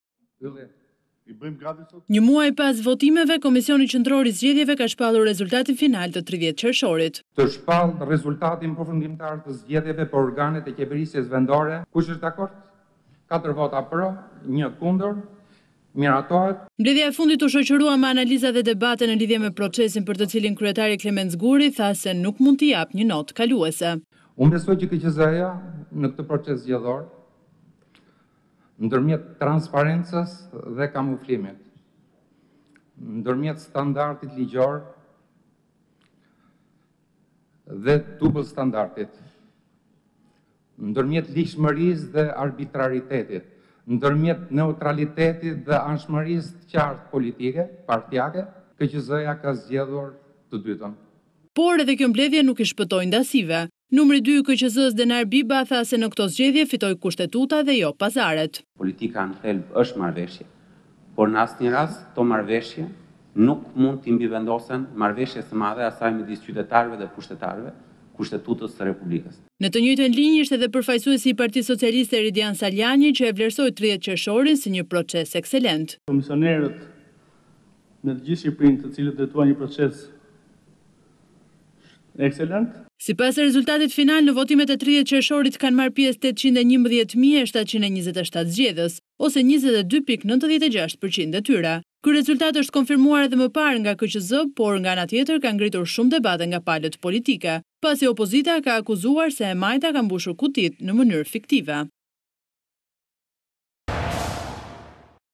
RTV Ora - Shpallet rezultati final i 30 qershorit, Klement Zguri: KQZ me standarde të dyfishta
Një muaj pas votimeve, Komisioni Qëndror i Zgjedhjeve ka shpallur rezultatin final për votimet të 30 qeshorit.

Mbledhja e fundit u shoqërua me analiza dhe debate ndaj proçesit, teksa kryetari i KQZ, Klement Zguri tha se nuk mund t’i jepte një notë kaluese

Klement Zguri: "Unë besoj se KQZ në këtë proces zgjedhor ndërmjet transparencës dhe kamuflimit, standardit ligjor dhe standardit të dyfishtë, ligjshmërisë dhe arbitraritetit, neutralitetit dhe anshmërisë së qartë politike partike KQZ zgjodhi të dytën.

Por ndryshe e ka parë procesin nënkryetari Denar Biba, tha se në këto zgjedhje fitoi Kushtetuta.

Klement Zguri: "Politika në thelb është një marrëveshje por në asnjë rast këto mrrëveshje nuk mund ti mbivendosen marrëveshjes së madhe mes qytetarëve dhe Kushtetutës."

Në të njëjtën linjë ishte dhe përfaqësuesi ligjor i PS Eridian Salianji, i cili e vlerësoi procesin "ekselent"

Sipas rezultatit final, në votimet e 30 qershorit kanë marrë pjesë  811.727 zgjedhës, ose 22,96 % e tyre, rezultat i konfirmuar edhe më parë nga kqz, por që ka ngritur shumë debate nga palët politike, teksa opozita ka akuzuar se e majta ka mbushur kutitë në mënyrë fiktive